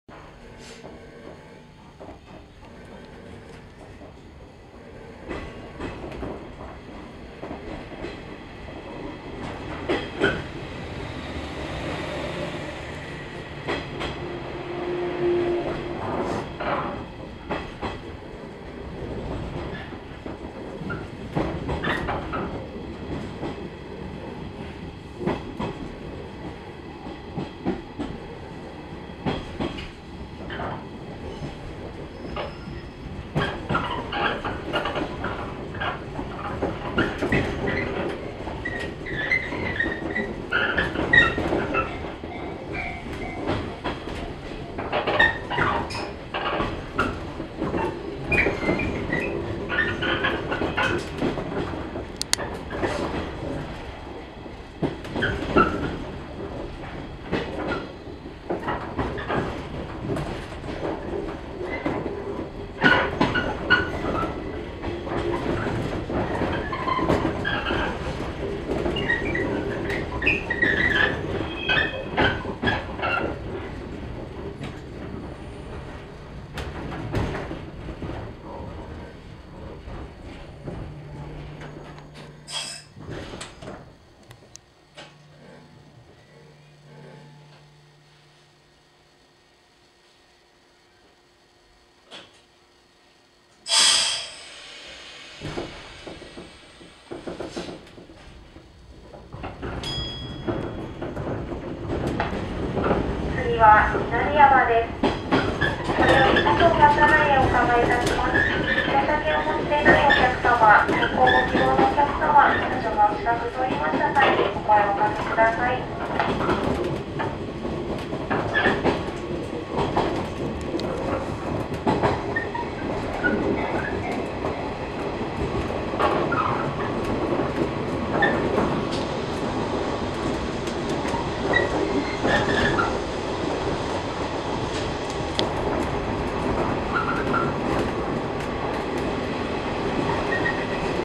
篠ノ井線姨捨駅スイッチバック
姨捨駅を出てスイッチバックして長野方面へと向かう篠ノ井線電車